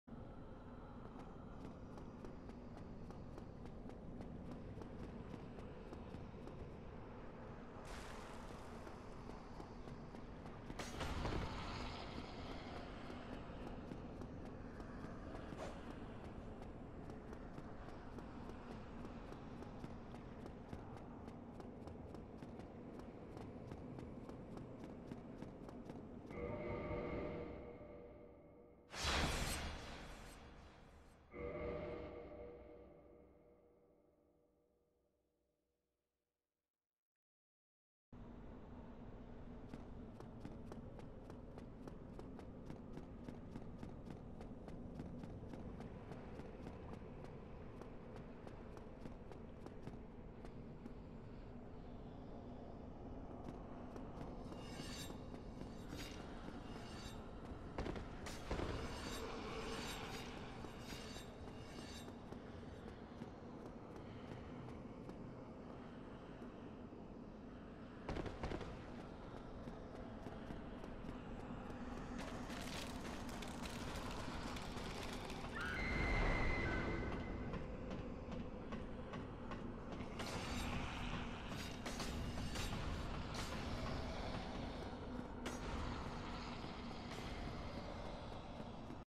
Ingward Rangeless
If you don’t want to quitout, wait at the place that I quitout for around 30-40 seconds and the ghosts will return to their spawn positions.